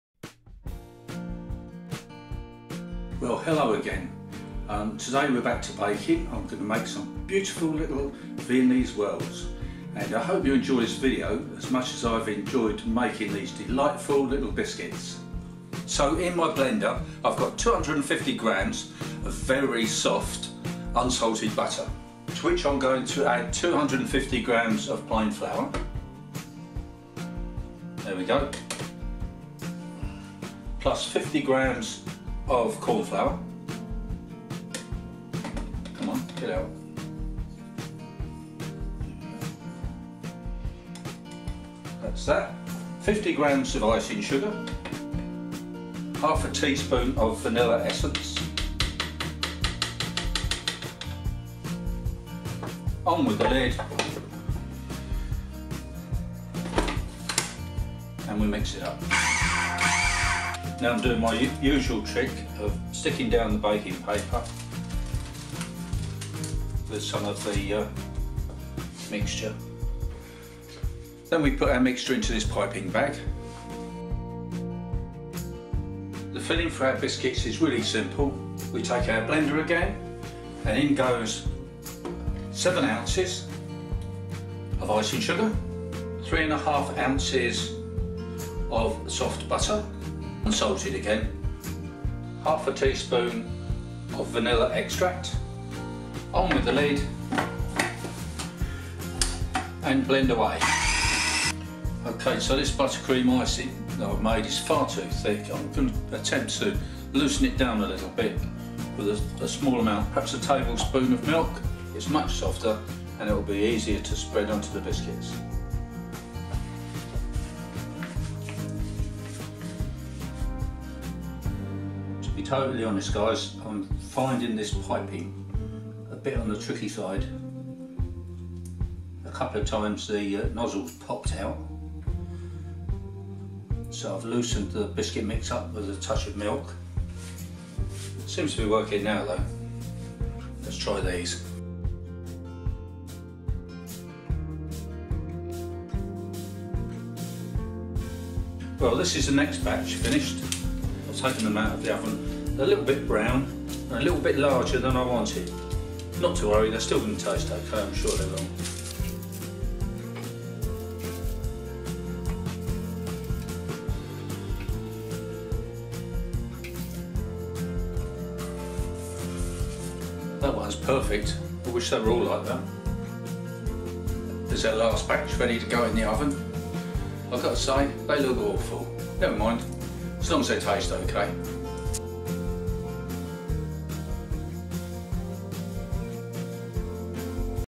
🍪🍪 VIENNESE WHIRLS
Viennese Whirls if you love sweet biscuits then this is for you.